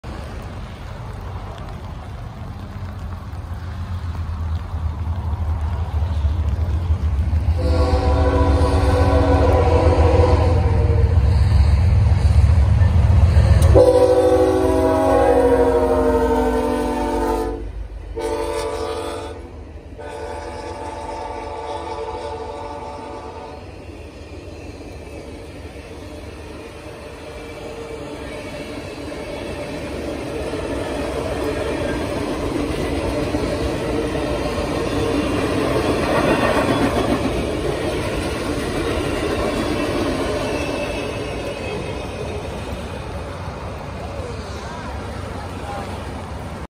NS Southern & Virginian HU’s lead 955
Gotten word of this train & I am so glad I was able to see this in person.